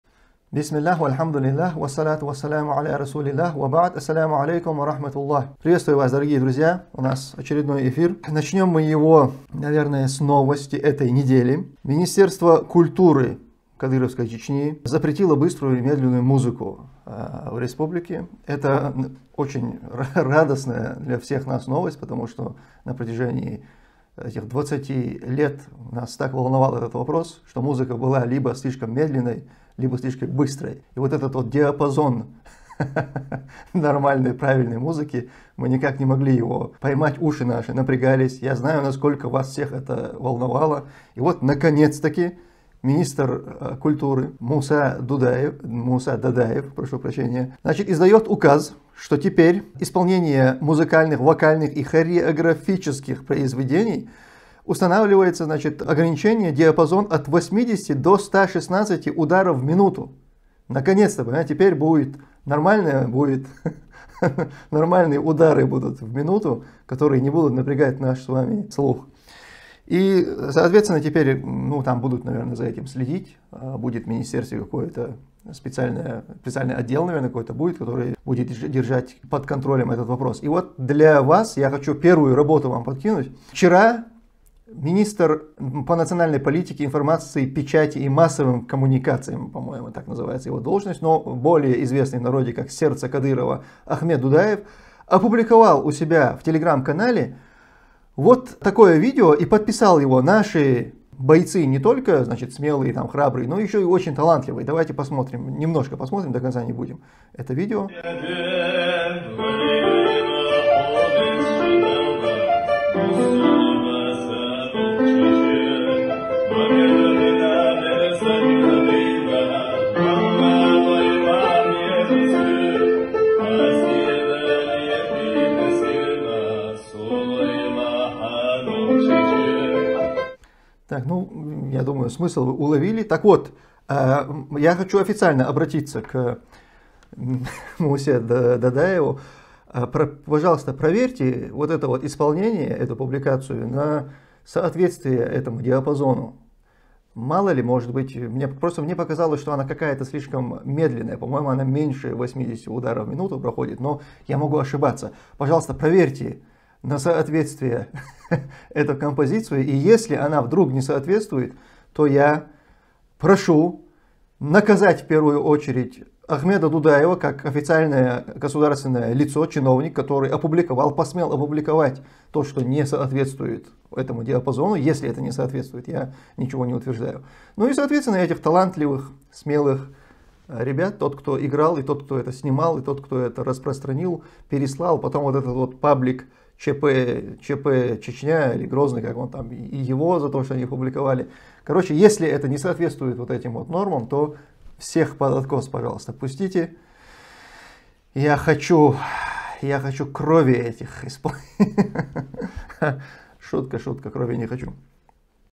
ЗАПРЕТ НА МУЗЫКУ В ЧЕЧНЕ ПРИЛЕТЕЛА ЖАЛОБА НА ТАЛАНТЛИВЫХ БОЙЦОВ
ЗАПРЕТ НА МУЗЫКУ В ЧЕЧНЕ  ПРИЛЕТЕЛА ЖАЛОБА НА ТАЛАНТЛТВЫХ БОЙЦОВ